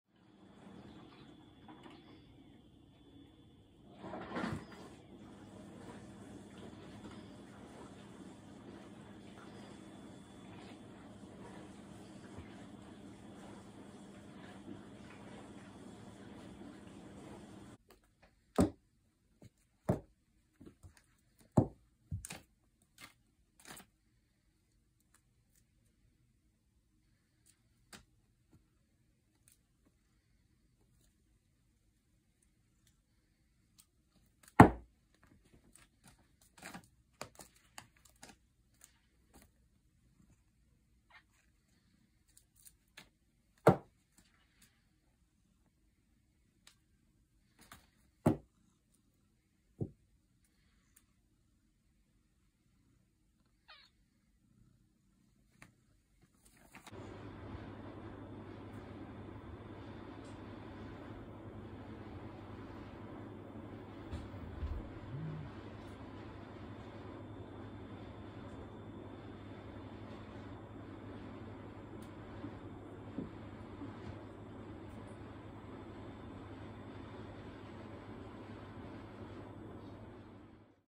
cat pro wrestling
My cats sometimes wrestle. They are so cute.